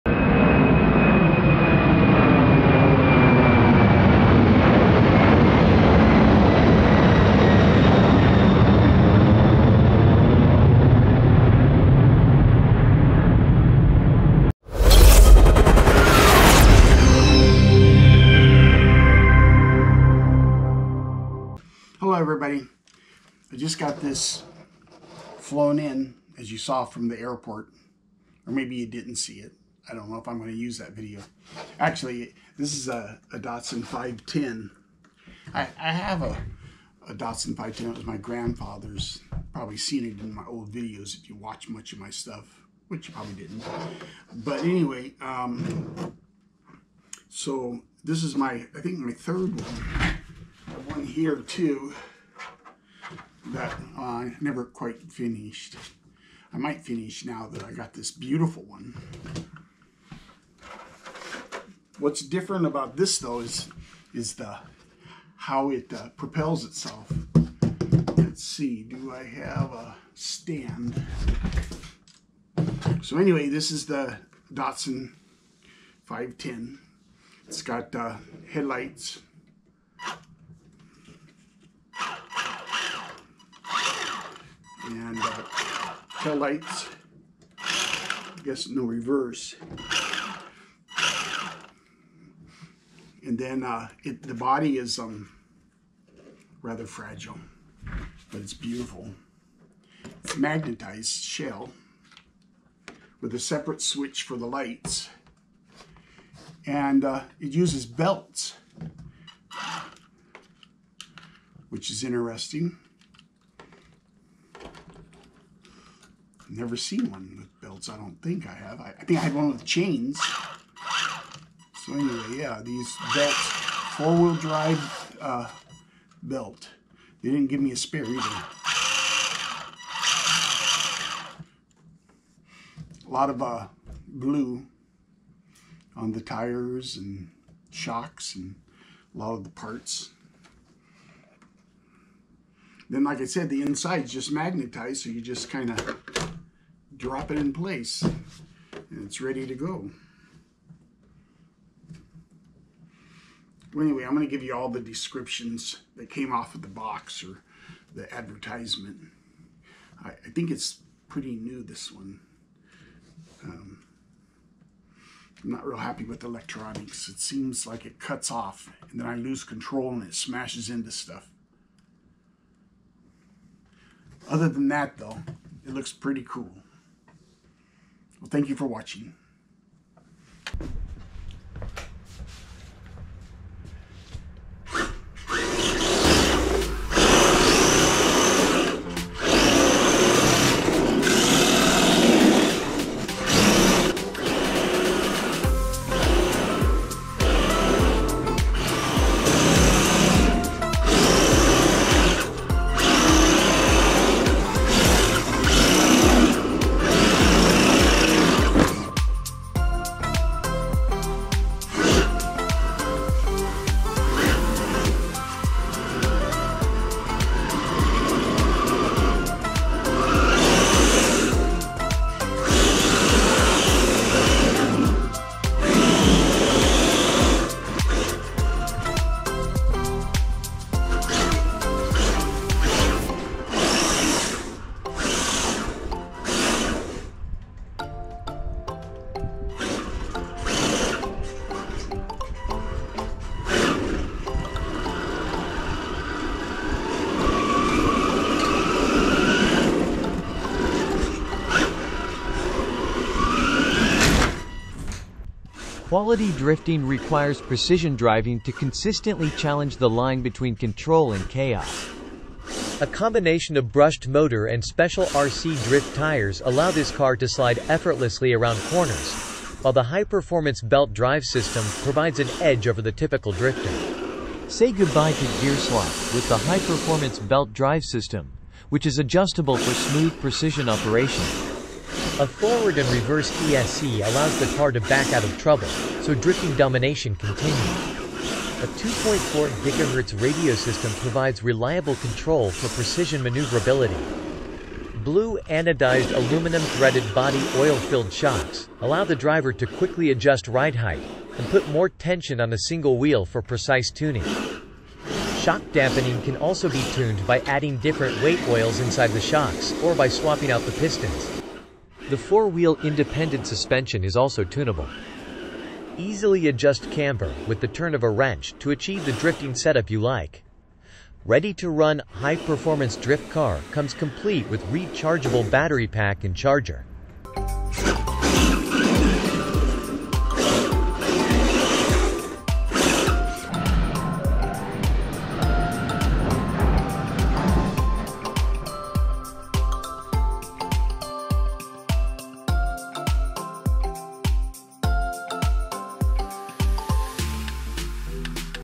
#549 DATSUN 510 BLUEBIRD RC
Staring:
RC 1/10 DATSUN 510 BLUEBIRD AWD DRIFT RTR W/ LEDS

PRODUCT DESCRIPTION:
RC DATSUN 510 BLUEBIRD DRIFT CAR

COMES 100% ASSEMBLED AND FINISHED

RADIO-BATTERY-CHARGER INCLUDED

FULL LED LIGHTS FRONT + BACK

MAGNETIC BODY MOUNTS, NO BODY HOLES

DRIFT WHEELS

HIGH PRECISION BELT DRIFT FULLY ADJUSTABLE CHASSIS CAN BE TUNED FOR DRIVING CONDITIONS.

METAL SUSPENSION CAN BE ADJUSTED.

2.4 GHZ RADIO-FULL BALL BEARINGS

NEW BUILD *READY TO RUN 100% BRAND NEW

Quality Drifting Requires Precision Driving To Consistently Challenge The Line Between Control And Chaos. A Combination Of Brushed Motor And Special RC Drift Tires Allow This Car To Slide Effortlessly Around Corners, While The High Performance Belt Drive System Provides An Edge Over The Typical Drifter.

Say Goodbye To Gear Slop With The High Performance Belt Drive System, Which Is Adjustable For Smooth Precision Operation.

A Forward And Reverse ESC Allows The Car To Back Out Of Trouble So Drifting Domination Can Continue. A 2.4GHz Radio System Provides Reliable Control For Precision Maneuverability.

Blue Anodized Aluminum Threaded Body Oil Filled Shocks Allow The Driver To Quickly Adjust Ride Height And Put More Tension On A Single Wheel For Precise Tuning. Shock Dampening Can Also Be Tuned By Adding Different Weight Oils Inside The Shocks, Or By Swapping Out The Pistons.

The  4 Wheel Independent Suspension Is Also Tunable. Easily Adjust Camber With The Turn Of A Wrench To Achieve The Drifting Setup You Like.

Ready To Run,  High Performance Drift Car Comes Complete With Rechargeable Battery Pack And Charger.